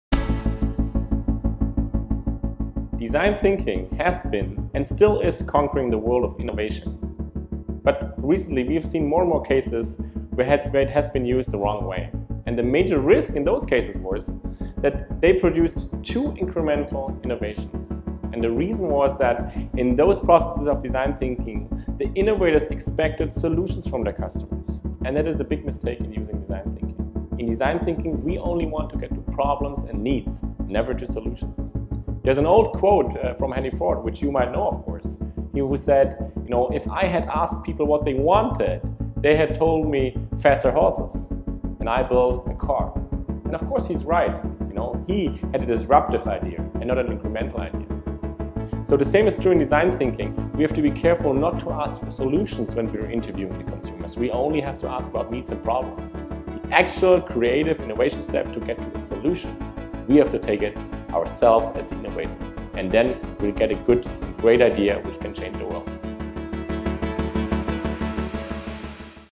Design Thinking in 60 Seconds
Design thinking is conquering the world - but there are some common pitfalls to avoid.